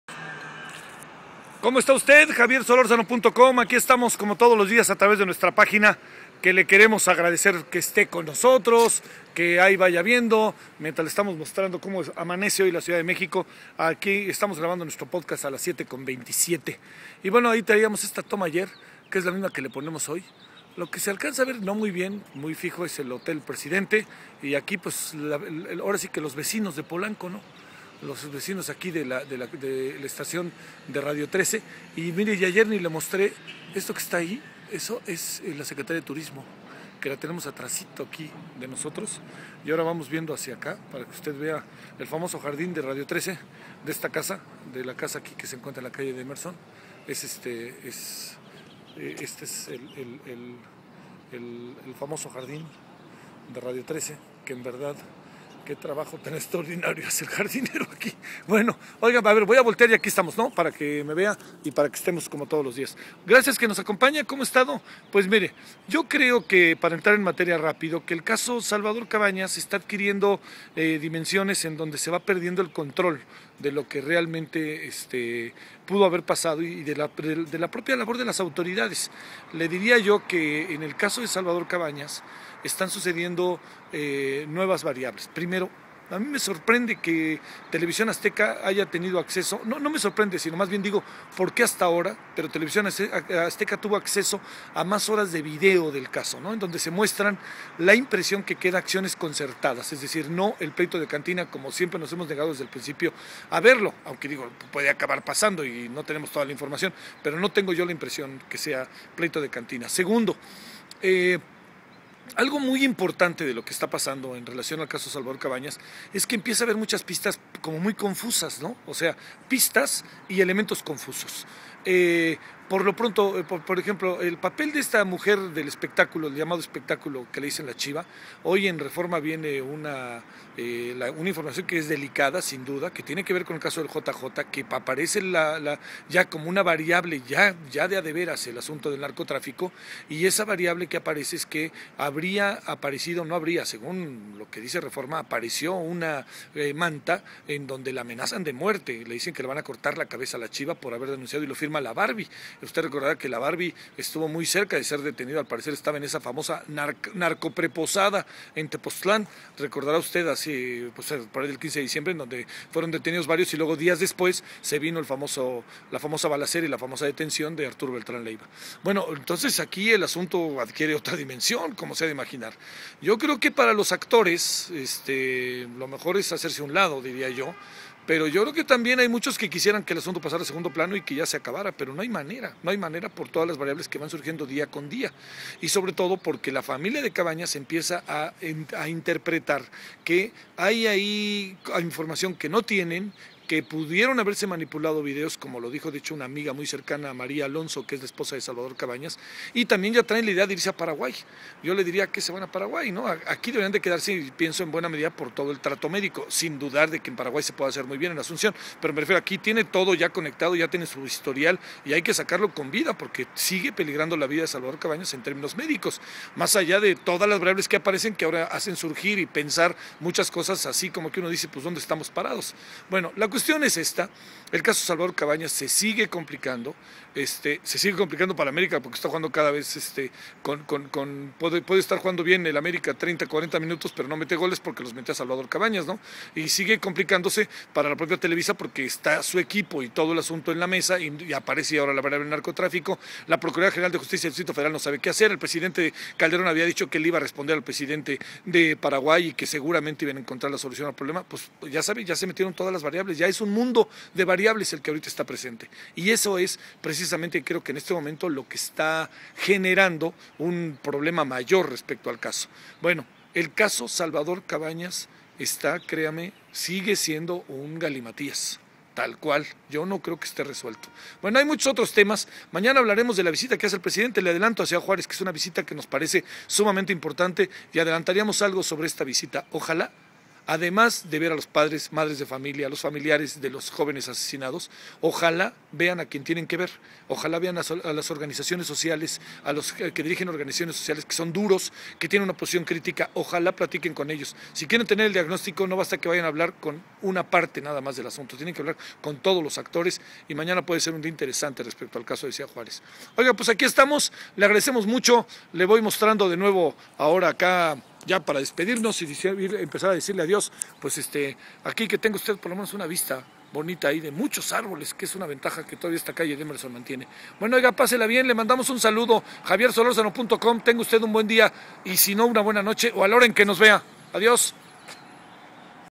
Confusión
Javier Solórzano habla sobre las nuevas variables que aparecen en el caso Salvador Cabañas, así como las pistas y elementos confusos que han surgido.